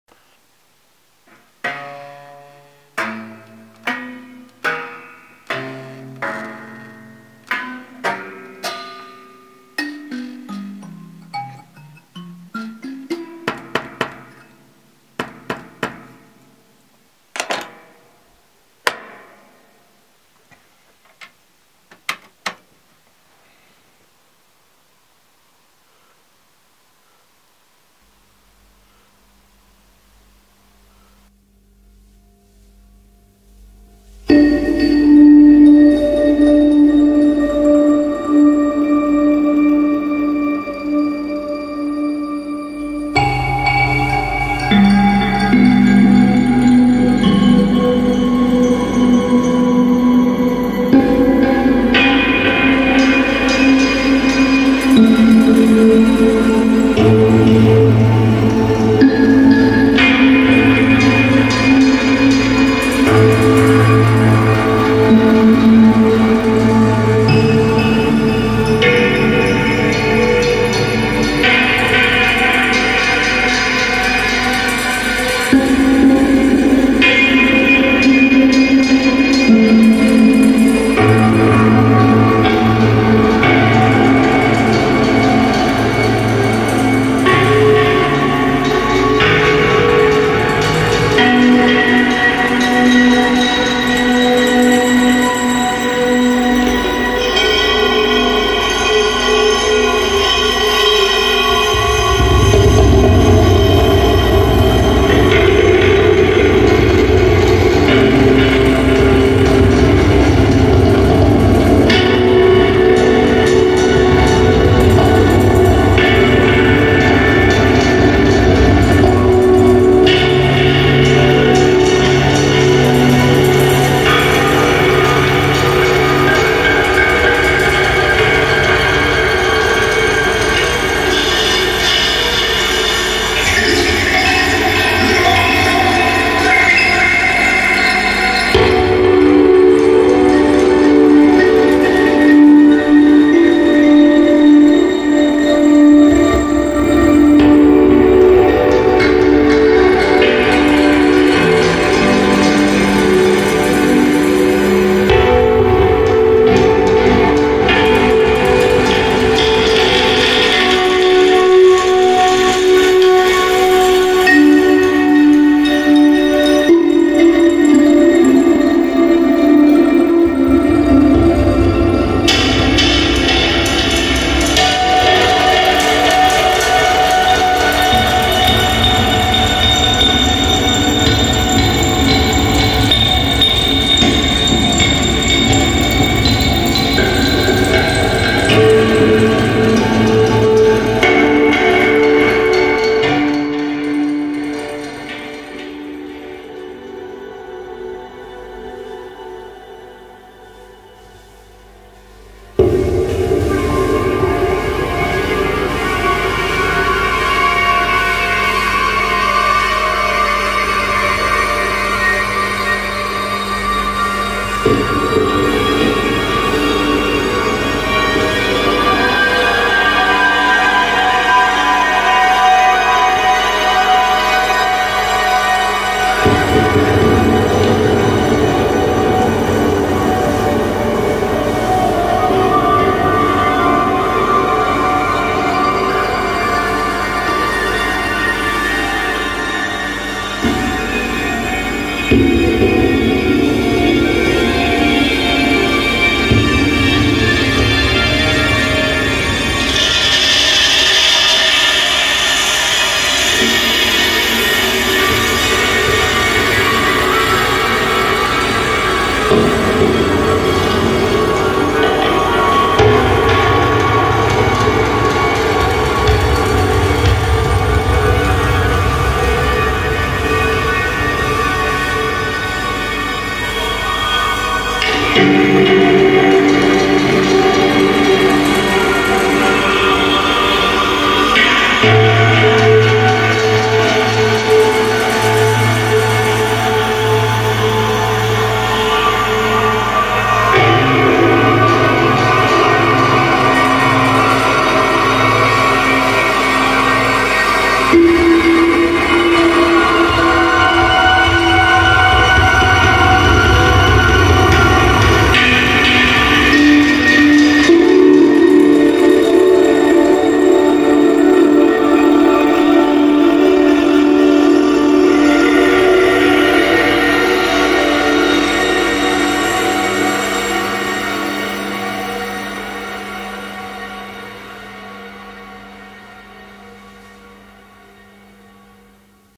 DIY Ambient box and Ableton Live
Ambient box is amplified by one piezoelectric sensor.
The springs of different thickness are tuned to different notes.
Ableton Live used to add some effects.